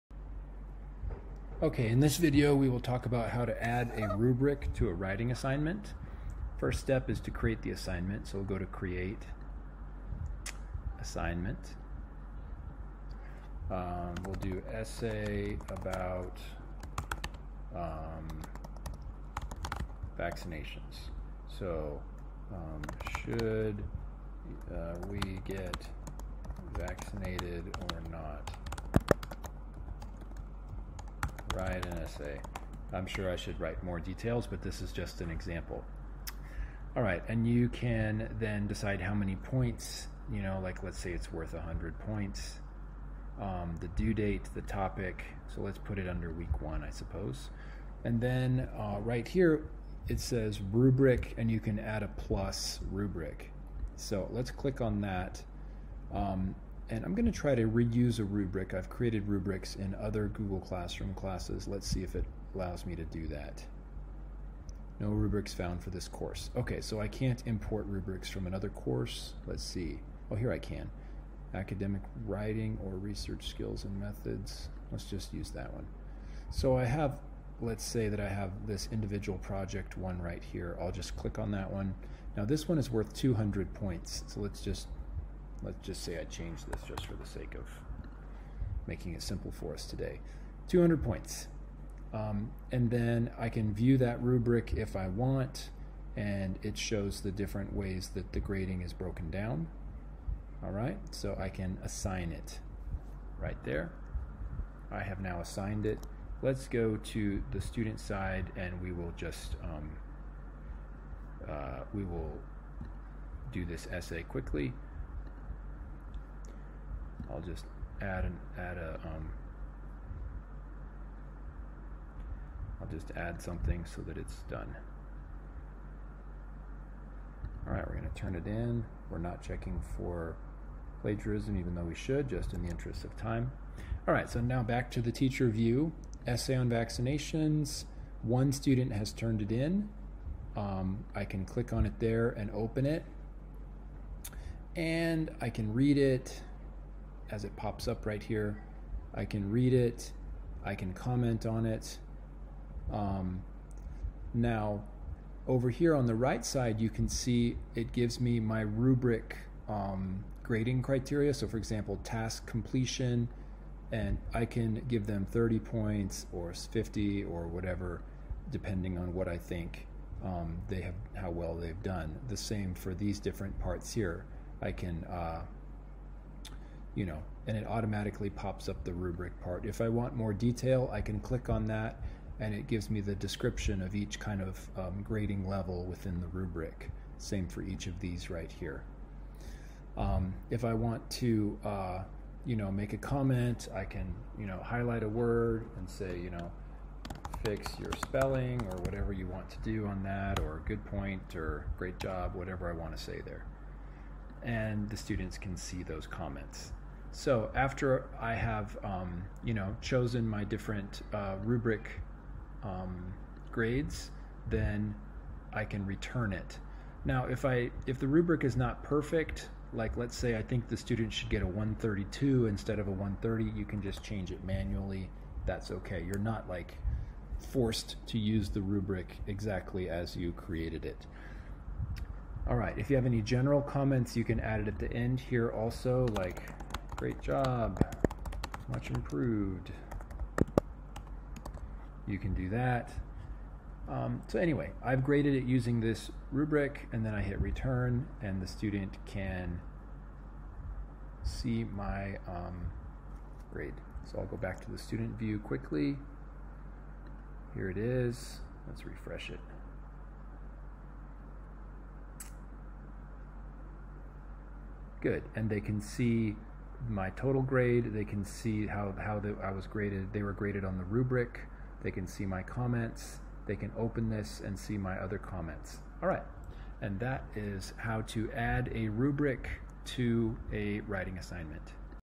How to add a rubric to a writing assignment in Google Classroom
How to add a rubric to a writing assignment in Google Classroom. You can reuse a rubric from your other courses in Google Classroom.